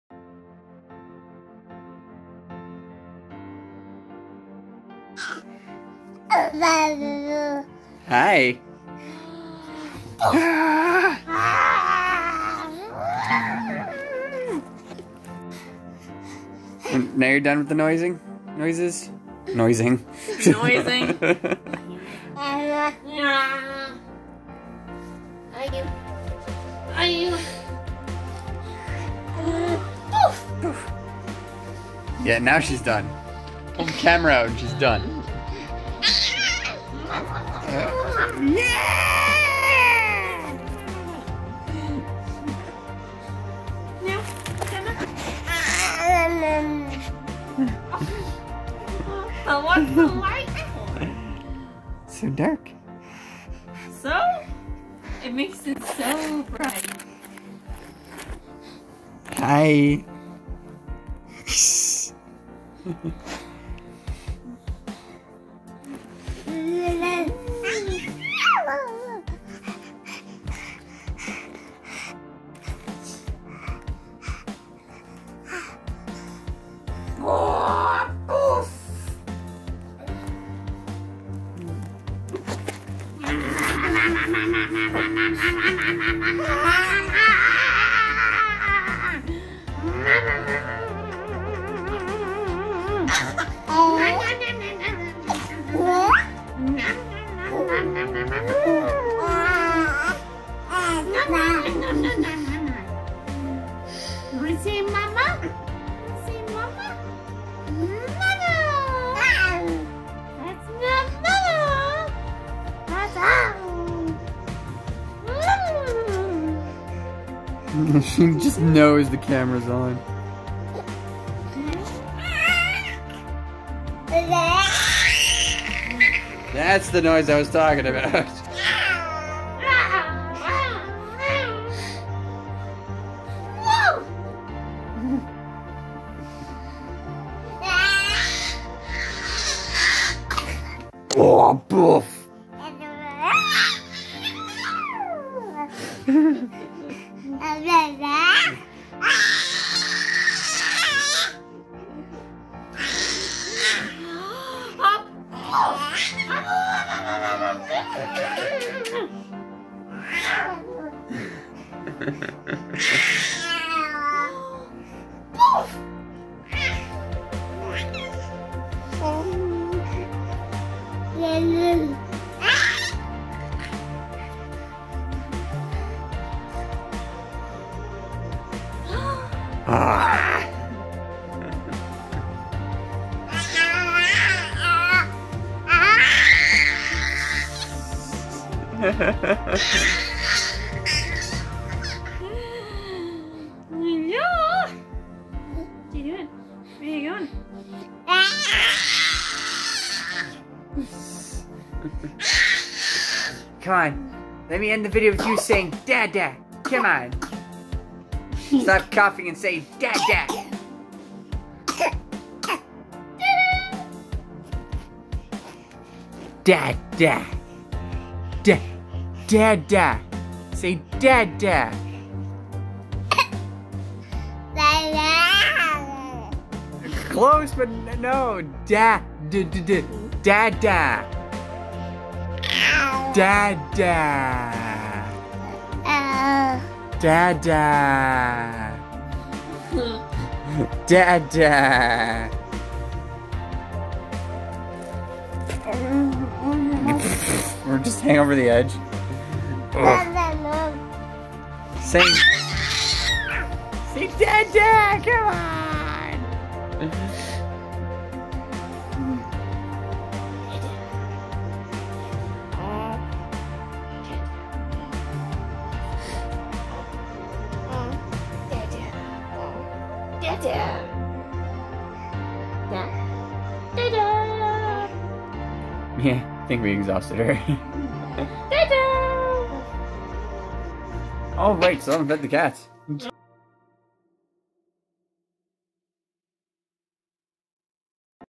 More Noises from Bridget
She is just a excited ball of fun!
I enhanced the brightness and lowered the contrast for better visuals.

RAM ON